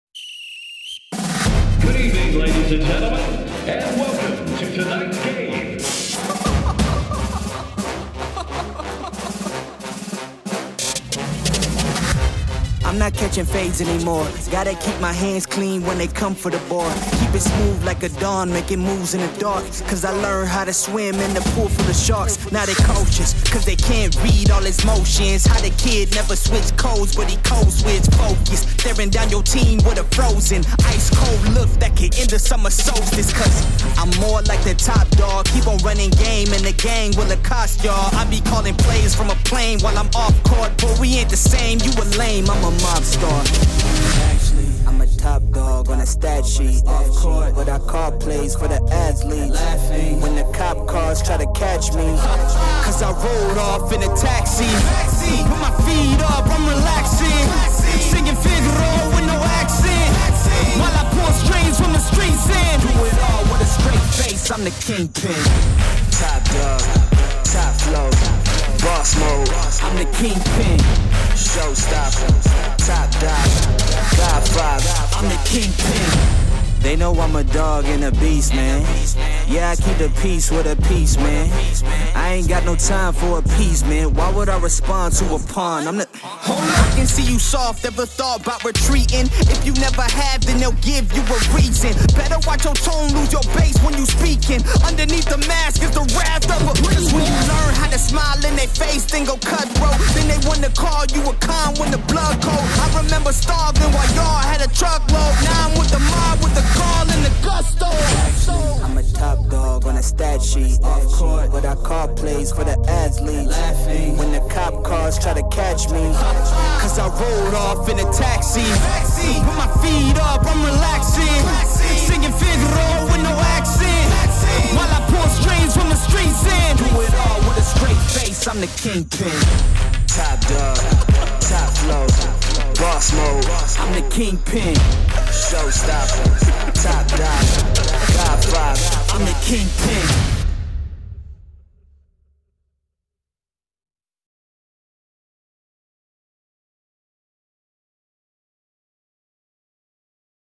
Sydney Ice Dogs vs Melbourne Ice & Mustangs | AIHL weekend 11-12/06/2022
Two awesome games 🏒🔥. Sydney Ice Dogs facing both teams from Melbourne Mustangs and Ice at the weekend 11th - 12th June 2022. The Australian Ice Hockey League.